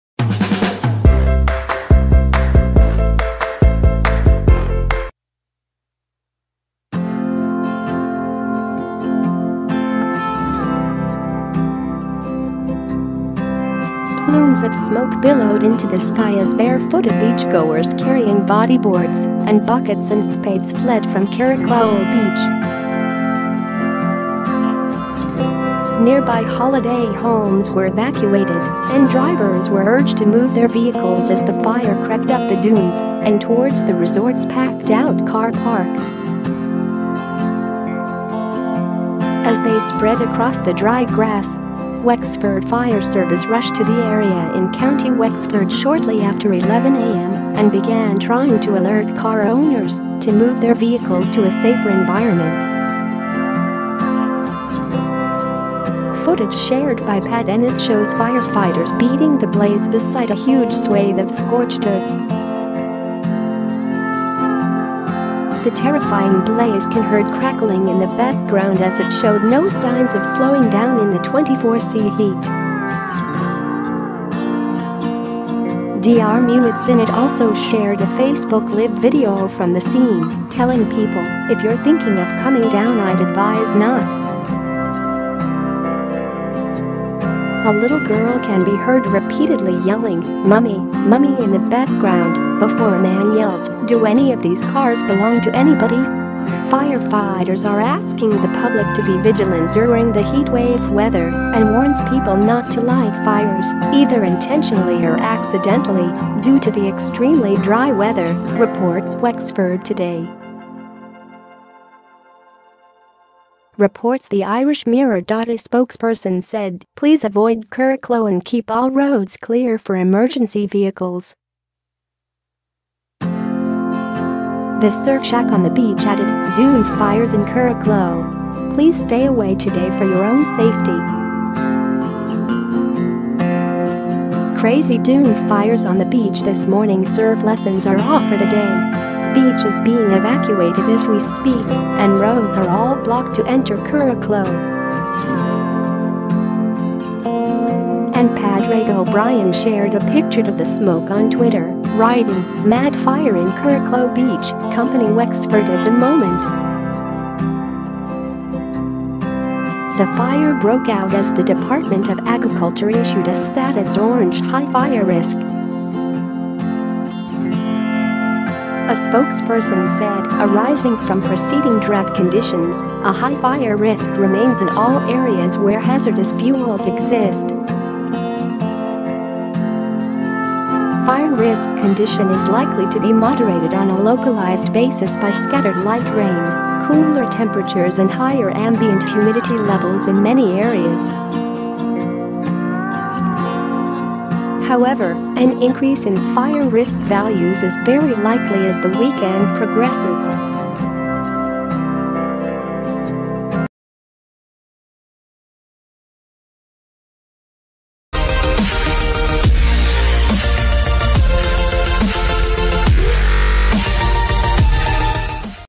Hundreds of holidaymakers evacuated from beach as huge fire breaks out in dunes - Daily News
Fire /form Hundreds of holidaymakers have been rushed to safety after a massive fire broke out in dunes at a beach in Ireland.  Plumes of smoke billowed into the sky as bare-footed beachgoers carrying bodyboards and buckets and spades fled from Curracloe Beach.  Nearby holiday homes were evacuated and drivers were urged to move their vehicles as the fire crept up the dunes and towards the resort's packed-out car park.   as they spread across the dry grass.  Wexford Fire Service rushed to the area in County Wexford shortly after 11am and began trying to alert car owners to move their vehicles to a safer environment.   Footage shared by Pat Ennis shows fire fighters beating the blaze beside a huge swathe of scorched earth.  The terrifying blaze can heard crackling in the background as it showed no signs of slowing down in the 24C heat.  Diarmuid Sinnott also shared a Facebook live video from the scene, telling people: "If you're thinking of coming down - I'd advise not."  A little girl can be heard repeatedly yelling: "mummy, mummy" in the background, before a man yells: "Do any of these cars belong to anybody?"    Fire fighters are asking the public to be vigilant during the ‘heatwave’ weather and warns people not to light fires, either intentionally or accidentally, due to the extremely dry weather, reports Wexford Today., reports the Irish Mirror.A spokesperson said: "Please avoid Curracloe and keep all roads clear for emergency vehicles."The Surf Shack on the beach added: "Dune fires in Curracloe. Please Stay away today for your own safety.    "Crazy dune fires on the beach this morning surf lessons are off for the day.  "Beach is being evacuated as we speak and roads are all blocked to enter Curracloe."   And Padraig O'Brien shared a pictured of the smoke on Twitter, writing: "Mad fire in Curracloe Beach, Co Wexford at the moment."  The fire broke out as the Department of Agriculture issued a Status Orange 'high fire risk.'  A spokesperson said: "Arising from preceding drought conditions, a High Fire Risk remains in all areas where hazardous fuels exist.  "Fire Risk condition is likely to be moderated on a localised basis by scattered light rain, cooler temperatures and higher ambient humidity levels in many areas. However, an increase in fire risk values is very likely as the weekend progresses."